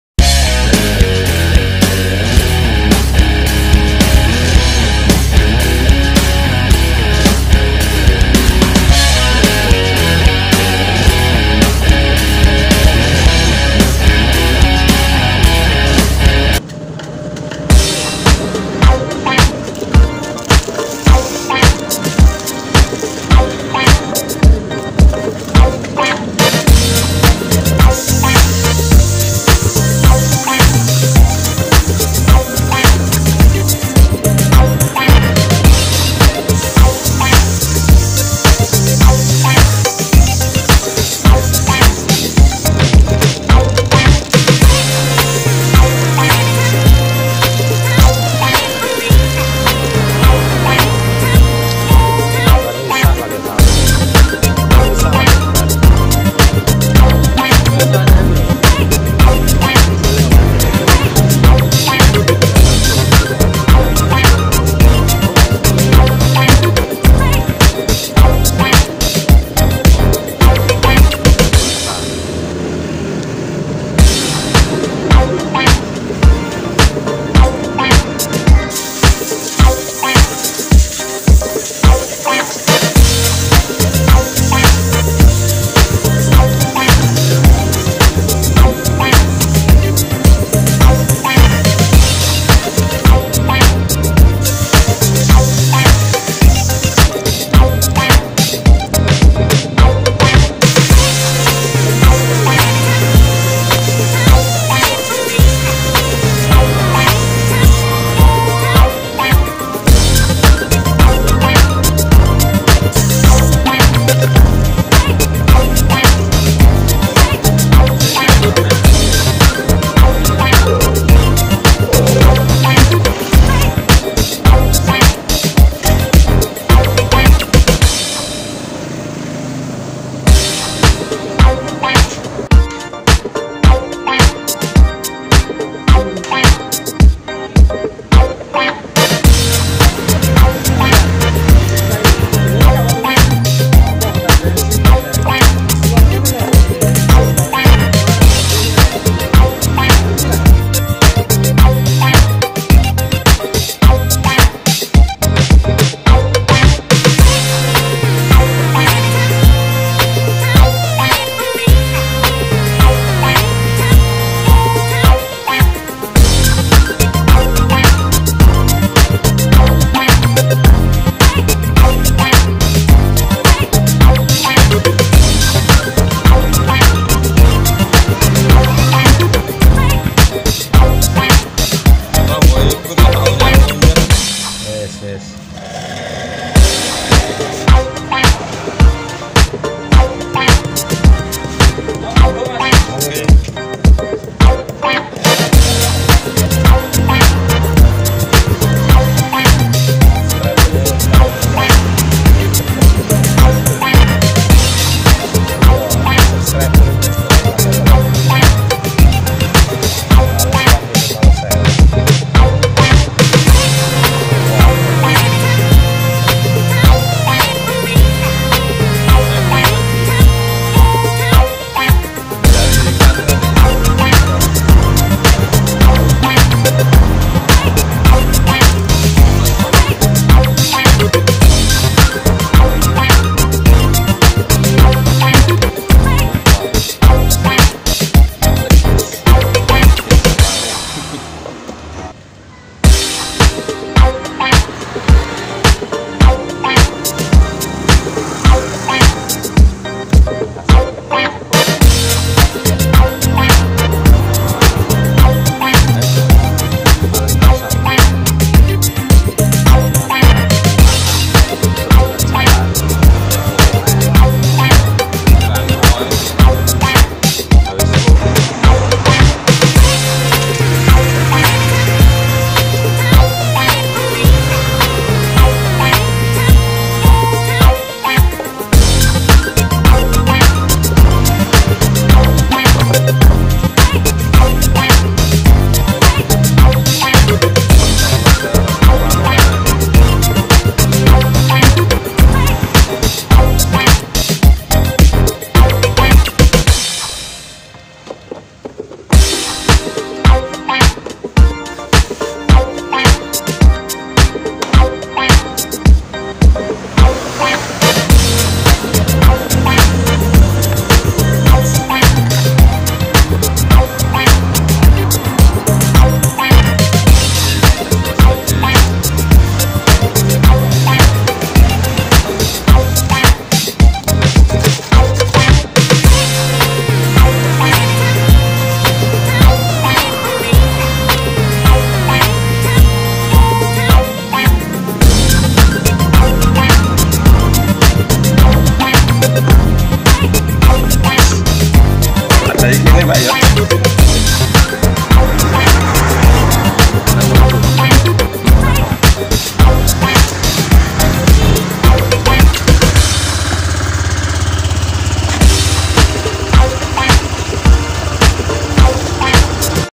Vario ganti aki | cara ganti aki motor Vario | BJM1TV
Vario ganti aki ,cara ganti aki Vario dengan aki kering merek Yuasa ytz6 motor Vario keluaran terbaru tidak mempunyai kick stater jadi cara untuk menghidupkannya dengan stater otomatis dg menggunakan aki kering tipe Yuasa ytz6v 12 volt